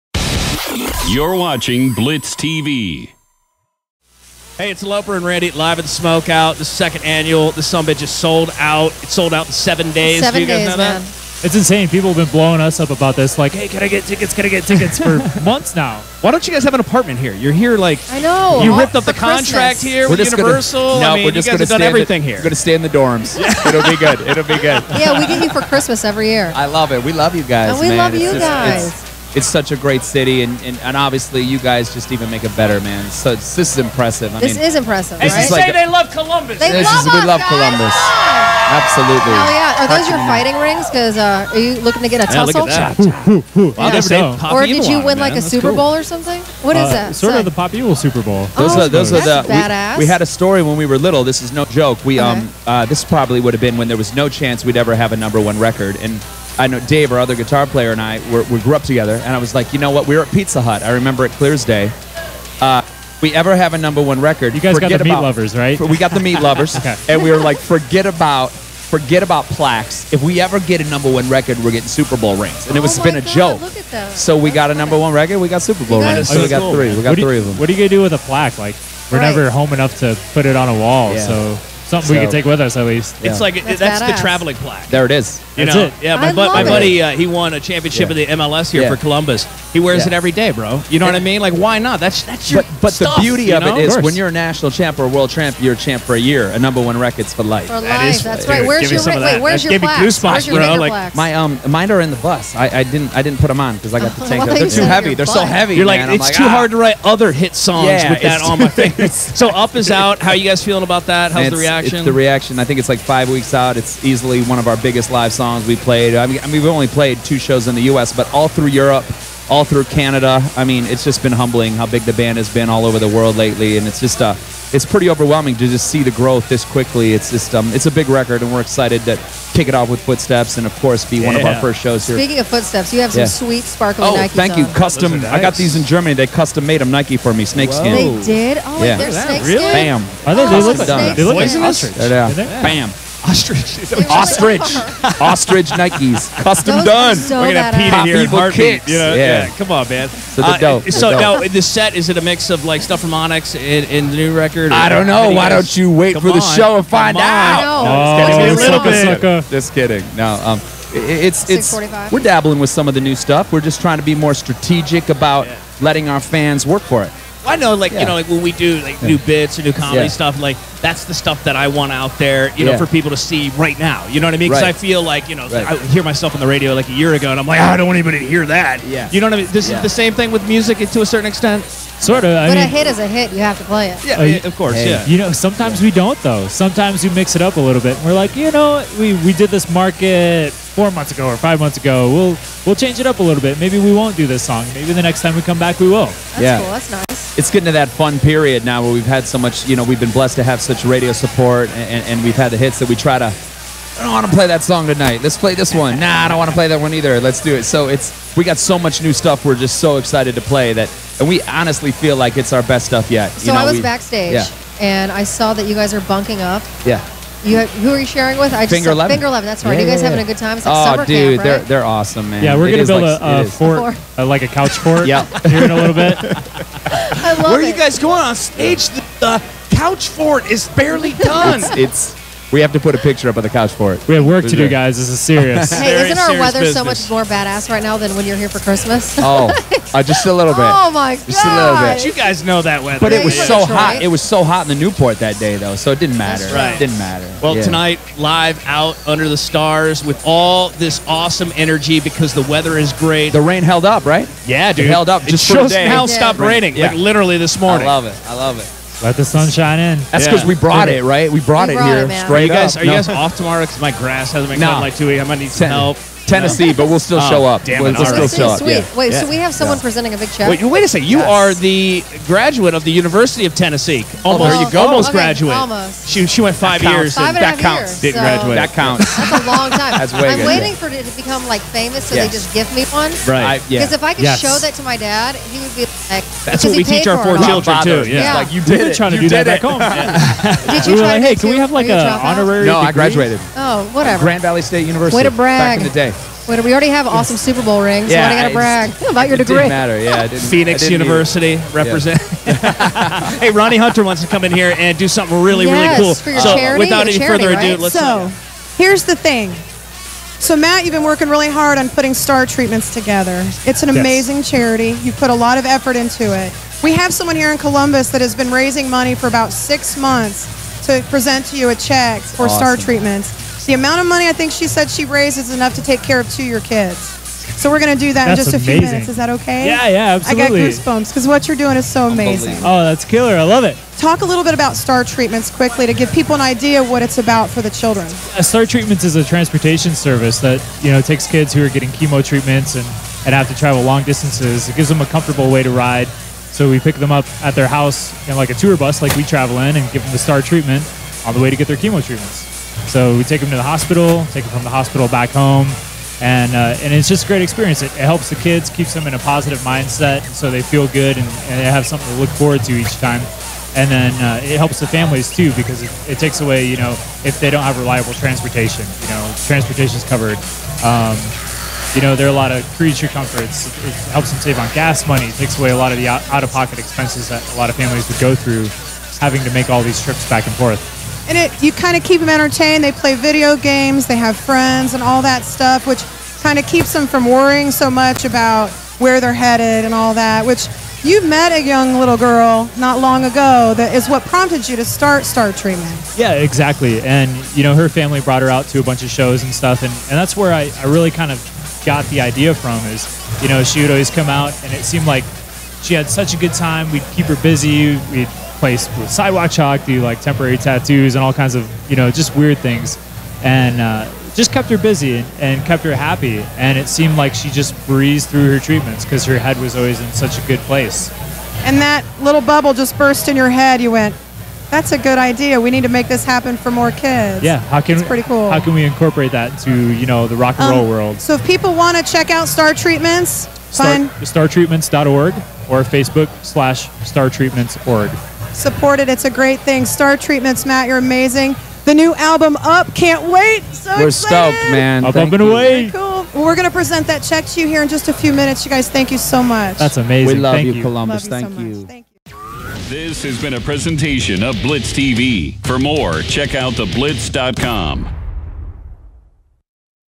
Pop Evil Interview - Great Summer Smokeout 2015
Pop Evil stop by the Blitz Stage to hang out with Loper and Randi during The Great Summer Smokeout 2015.